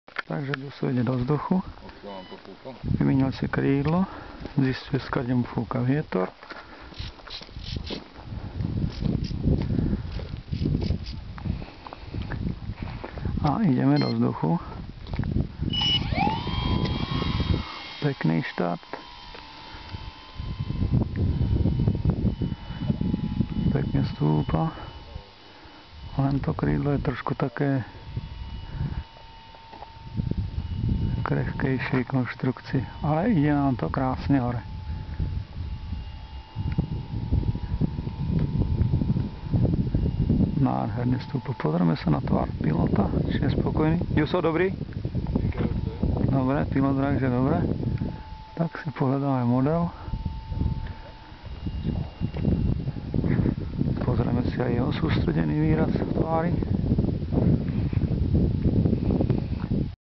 EVO6 B
Môj model EVO6 so strieďákom a vrtuľou 10x6 s osemčlánkom TT1100 mAh.
Čas letu: 1:00

V závere letu vetroň visí...